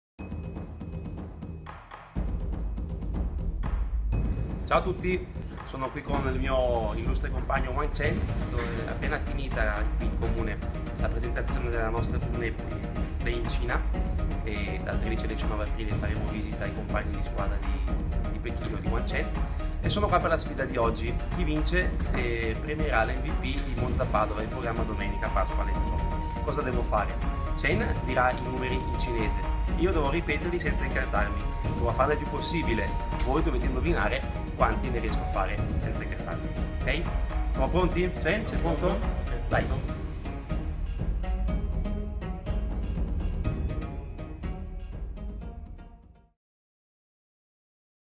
SFIDATIBE5
Sfida il Capitano Simone Tiberti e vieni a premiare l'MVP del match del 5 aprile 2015 Vero Volley vs Tonazzo Padova